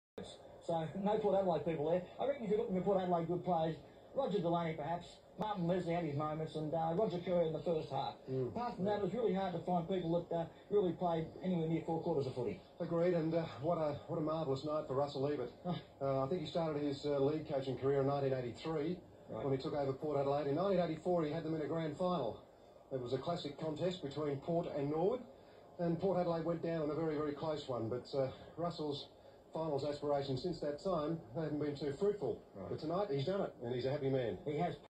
1988 Woodville V Port Adelaide - Kids in the background.
Wow wee sometimes the internet can unearth some strange, random and wonderful things.

33 years after the event, I came across the 1988 Escort Cup Grand Final. Over 31,000 SANFL Footy fans came out to Football Park at West Lakes to watch that game and I went with my old school mate / fellow Woodville fan Graham Peckman.

Woodville had never won a senior SANFL Premiership, and we were there to witness history. It was an incredibly special night, were we 15 years old and my Dad dropped us off and picked us up after the game.

My Dad and his brothers are former Woodville players and my Grand Dad was a long serving club trainer, needless to say our family were proudly green and gold.

Woodville : 14.12.96
Port Adelaide: 7.9.51

At the end of the Ch10 TV game commentators Peter Marker and Graham Campbell were doing the game review and Graham Peckman (L) and I (R) were the kids in the background on the other side of the window waving our scarfs in celebration...

How good was the SANFL back in those days, footy in SA was in great shape and the game was played tough.